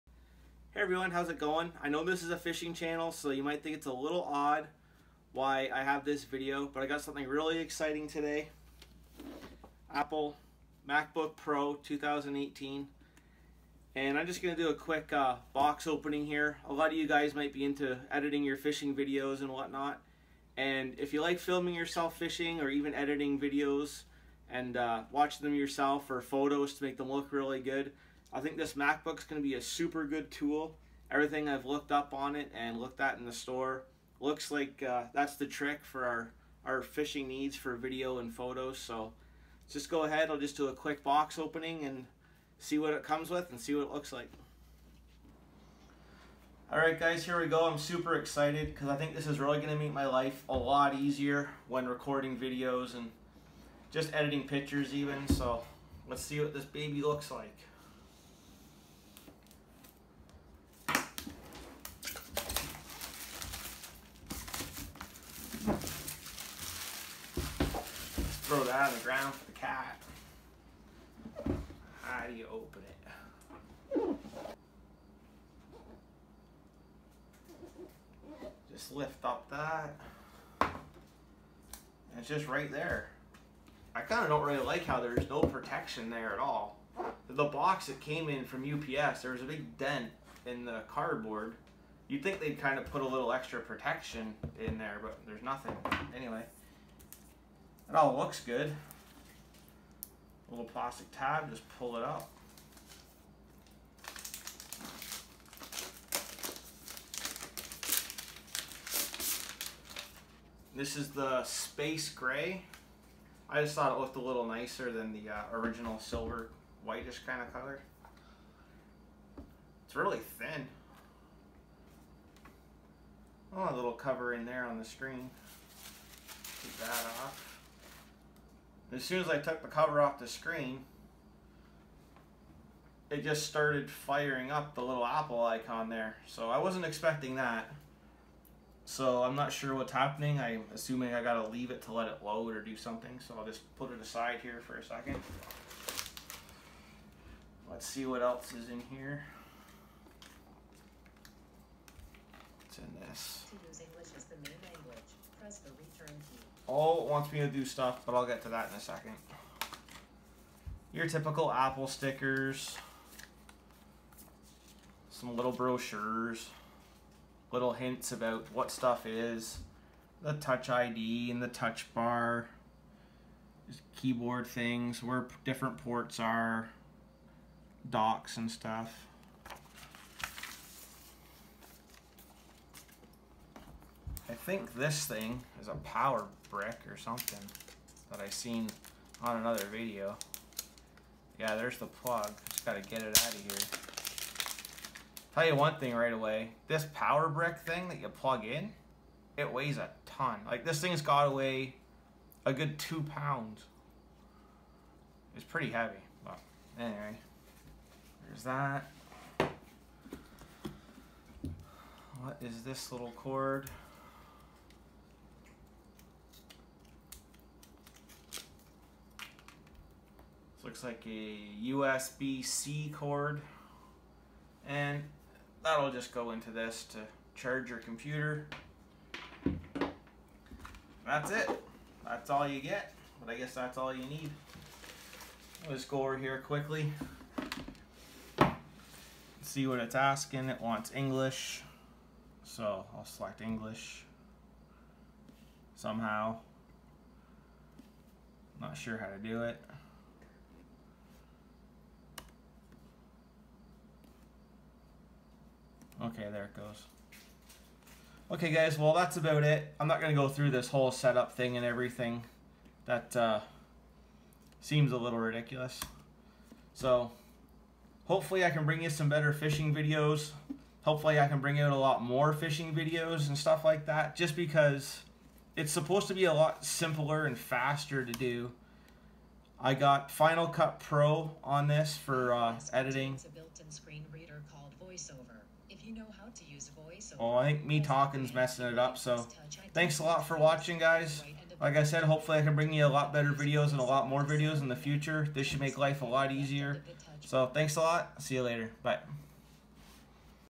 MacBook Pro 2018 unboxing
I open the new 2018 Mac Pro that I will be using for fishing vids
this is the first vid I edit on Mac and I'm already so happy with it 😁
what a difference from windows.